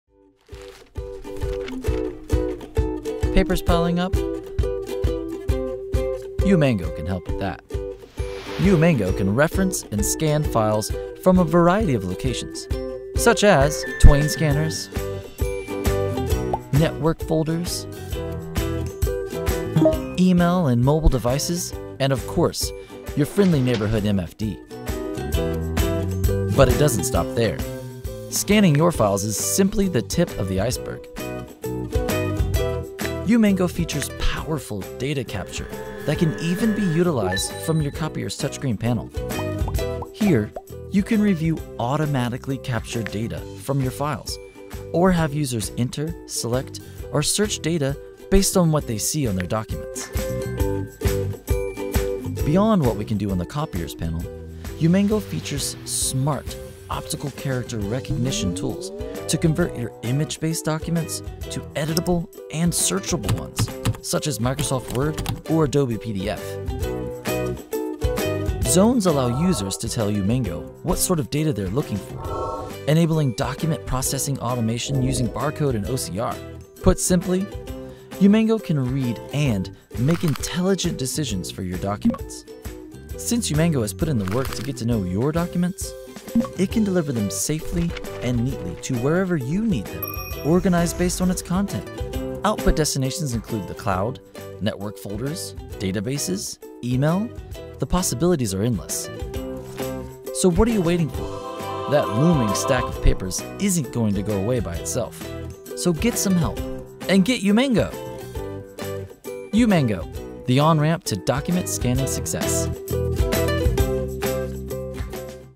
Umango: Scanning & File Conversion
Umango is a professional, easy-to-use document batch scanning application for those who need to scan, index & store files efficiently - exclusively distributed across Europe by iTS.

Umango provides a powerful batch scanning solution for document conversion and routing. With seamless connectivity into products like Microsoft SharePoint, HP Trim, Alfresco and a world of other solutions, Umango provides a powerful foundation to build your document imaging requirements around.

The speed and accuracy in document processing that Umango delivers, allows your business to access information quickly and easily.